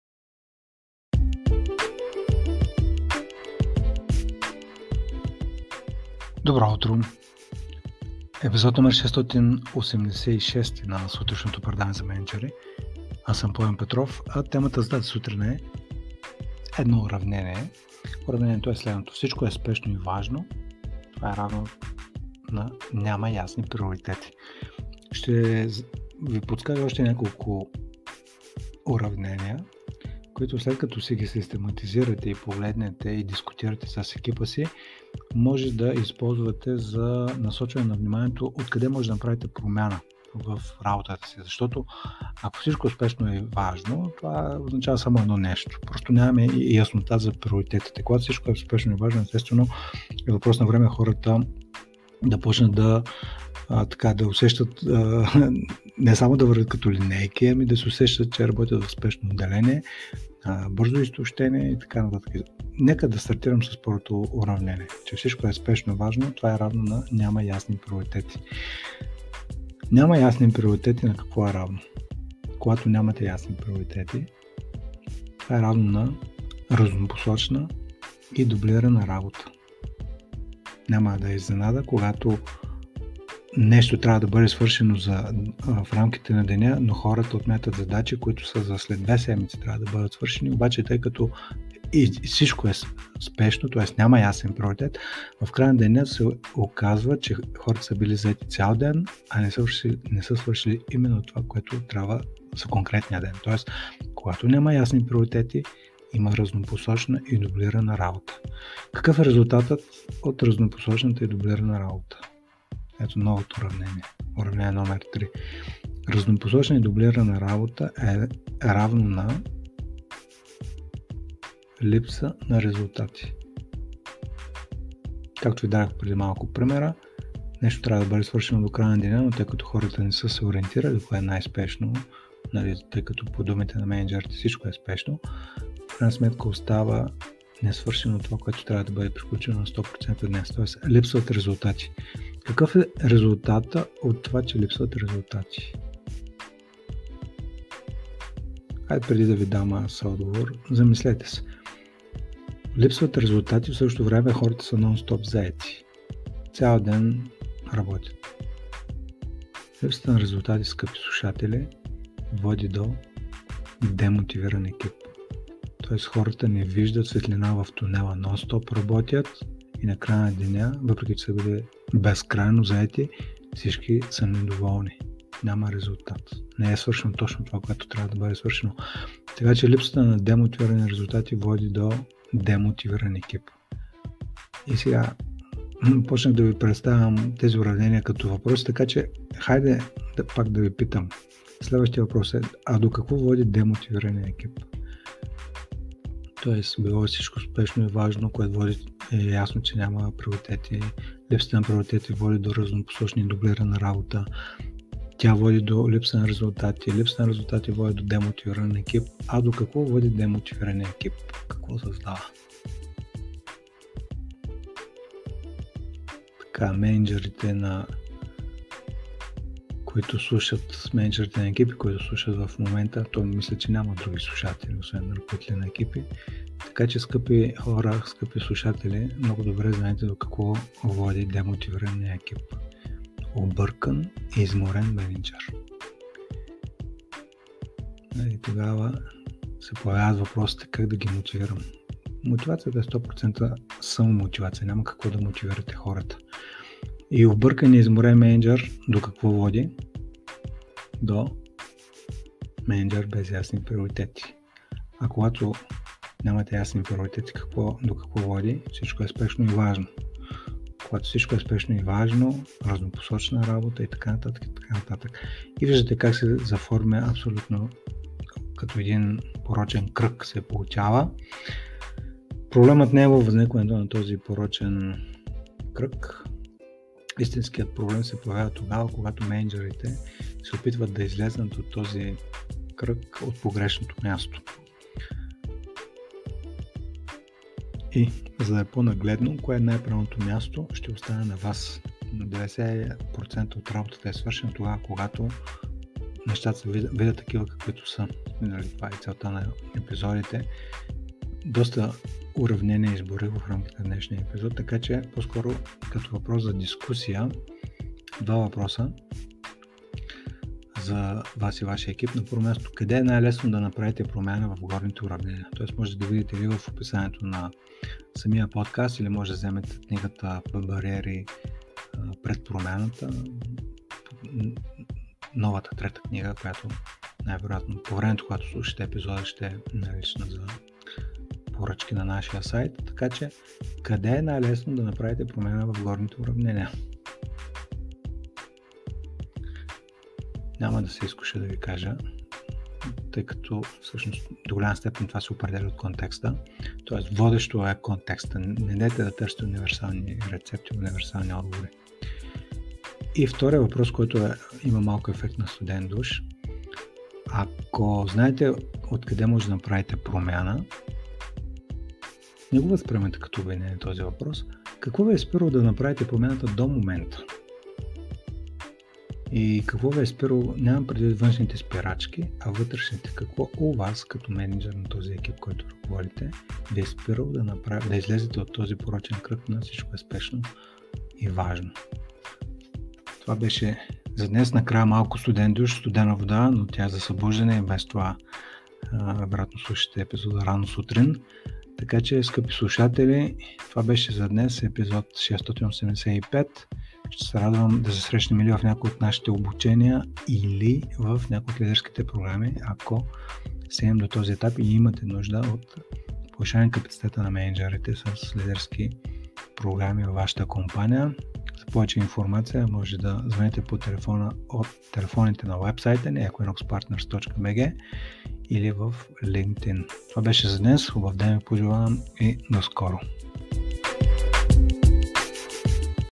Всичко е спешно и важно = Няма ясни приоритети | Епизод 686
Ще изброя няколко уравнения, които могат да ви помогнат да погледнете на работата си отстрани и да я подобрите. Ето ги и тях:

Всичко е спешно и важно = Няма ясни приоритети

Няма ясни приоритети = Разнопосочна и дублирана работа

Разнопосочна и дублирана работа = Липса на резултати

Липса на резултати = Демотивиран екип

Демотивиран екип = Объркан и изморен мениджър

Объркан и изморен мениджър = Всичко е спешно и важно

         Проблемът не е във възникването на този порочен кръг. Истинският проблем се появява тогава, когато мениджърите се опитват да излезнат от цикъла от погрешното място. И малко по-късно се оказват отново на мястото, откъдето са тръгнали.

         1) Къде е най-лесно да се направите промяна в горните уравнения?

         2) Какво и ви е спирало да направите промяната до момента? (не външните, а вътрешни спирачки)

3-месечна програма за развитие на ключови мениджърски умения (по заявка на една компания с мениджъри в групи до 12 души)